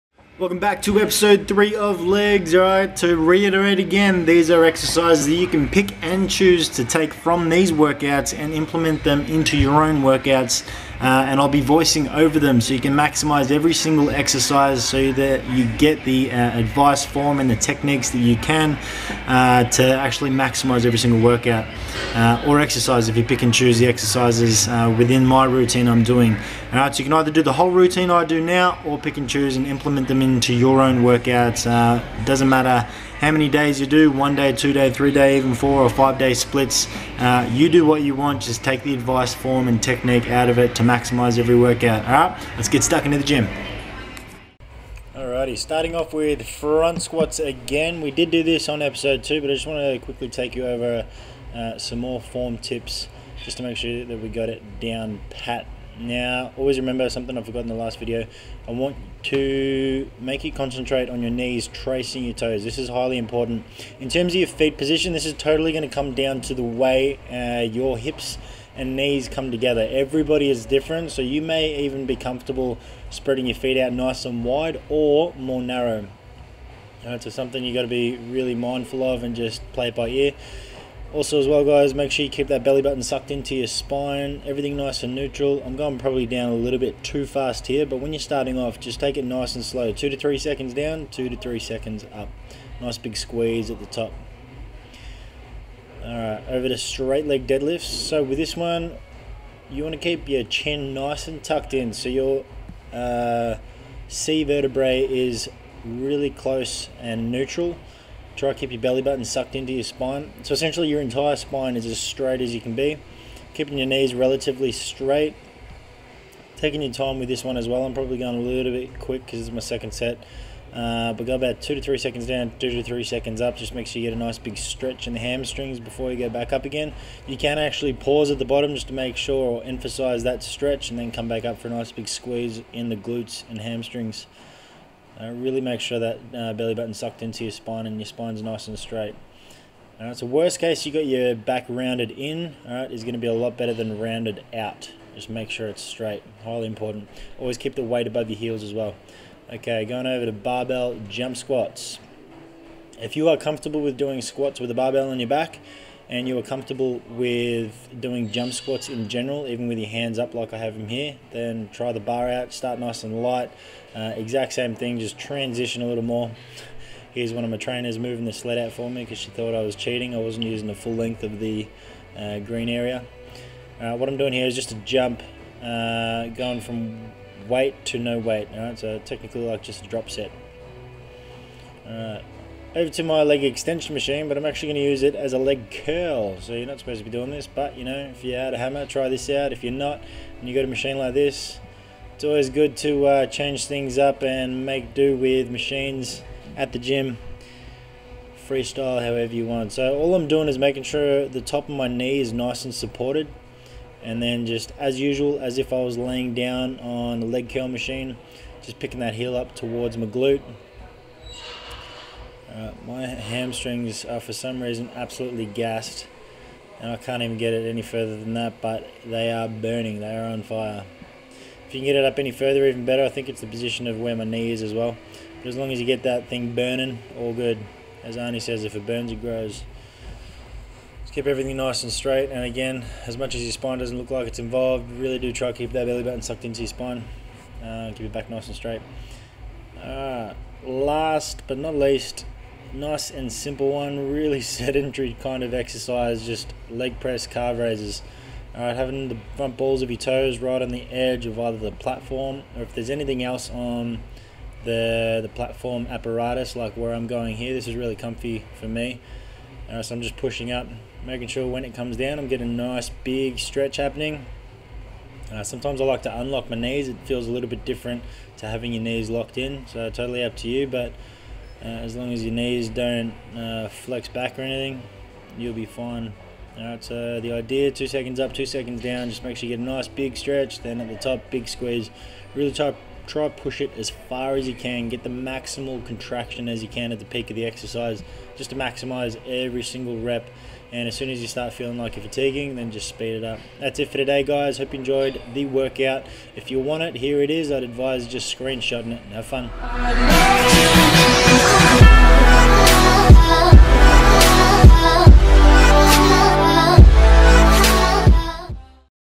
Legs Day Form Tips Ep 3
Episode 3 of the Exercise Form Tips series - Leg Day.
Again this is so you can either pick and choose any exercises you wish or use the entire workout to maximize your workouts with the added benefit of some tips and advice on form. Enjoy!

For your FREE 7 Day Trial at Hammrr Fitness go